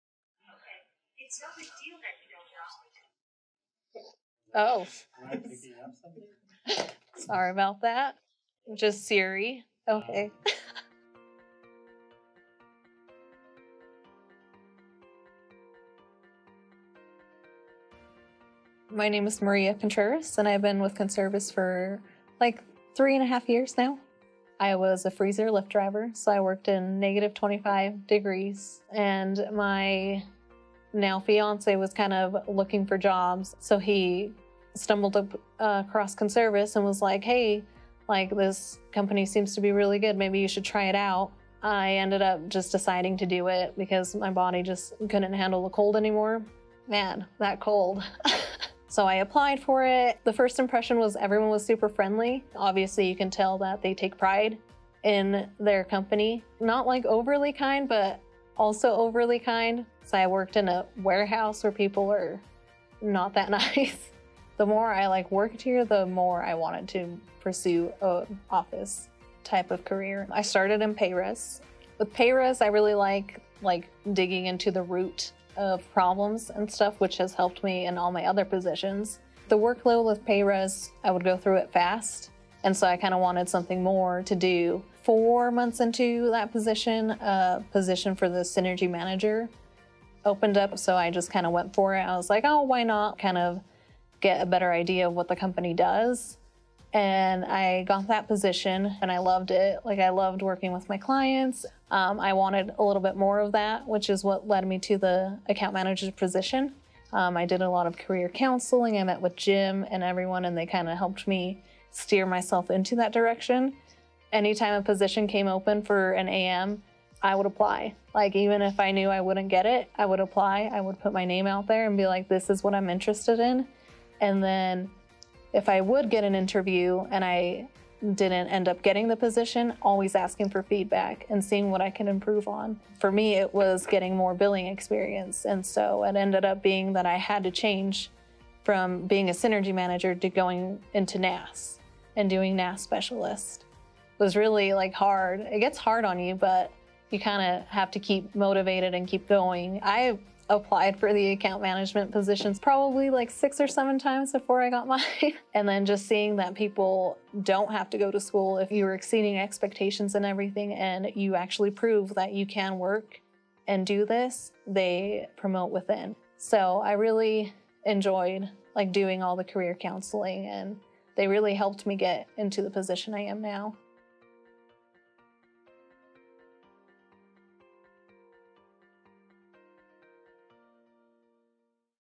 The Conservice Journey: Maria Contreras
"People don't have to go to school. If you are exceeding expectations, and you actually prove that you can work and do this, they promote within." -- Maria Contreras, Conservice Associate Account Manager

Apply online today!